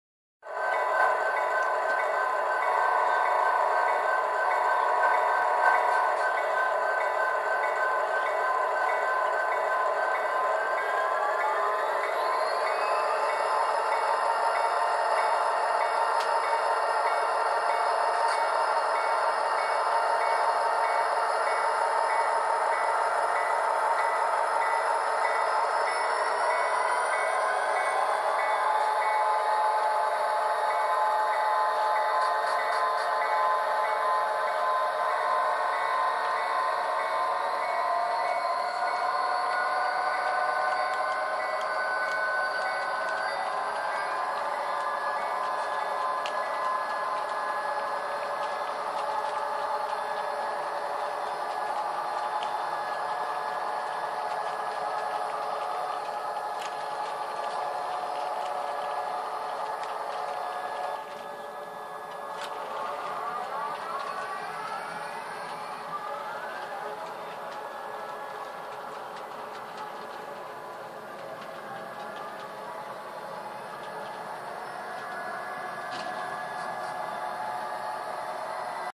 NS Freight
After the track inspector and the other NS train were clear NS 7266 & 7305  throttle up and leave Danforth.